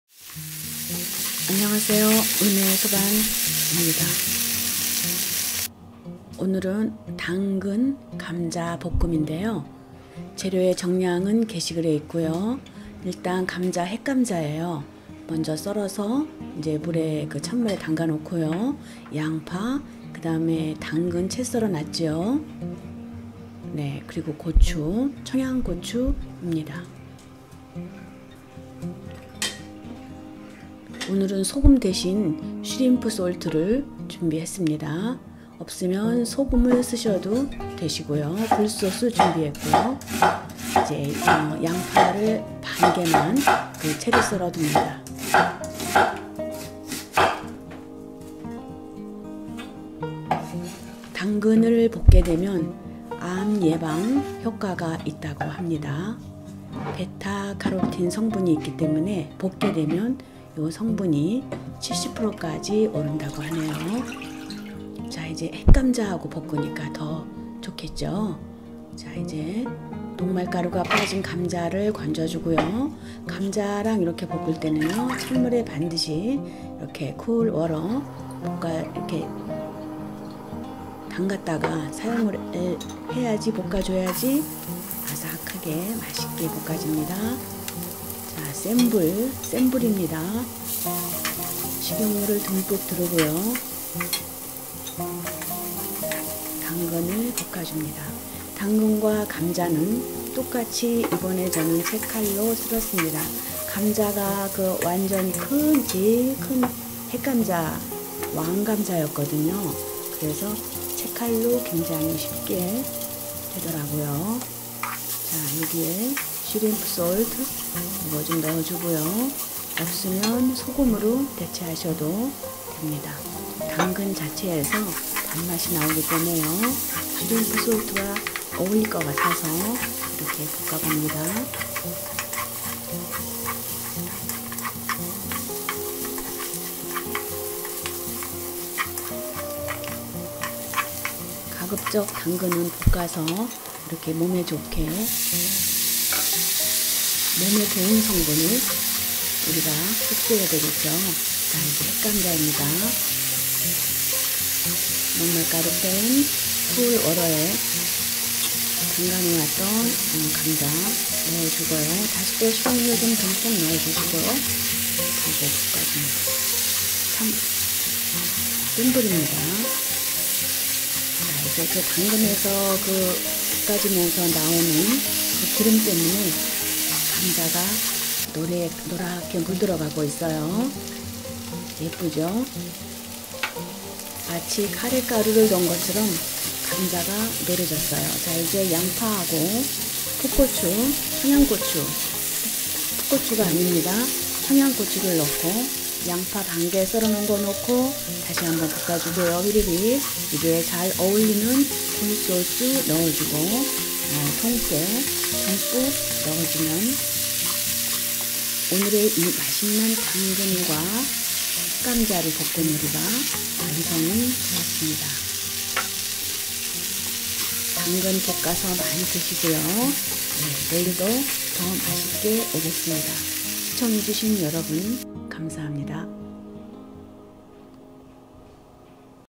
*당근감자볶음*(집밥) 당근을 볶아야 되는 이유(?)
햇감자 채썰어 찬물에 담가놓고,당근채썰고,양파 고추 썰고 센불에 식용유넣고 당근먼저 볶다가 감자를 볶고 노래지면,양파,고추 넣어볶고 소금으로 간을하고, 굴소스,깨넣고 마무리 ~센불 중불조절, 당근볶아서 많이 드세요 건강에 좋으니까~

재료
왕감자 1개
양파 1/2개
당근1개
청양고추 2개
쉬림프쏠트(혹은 소금)
깨 2수저
굴소스 1/2수저
식용유 적당
찬물 1000ml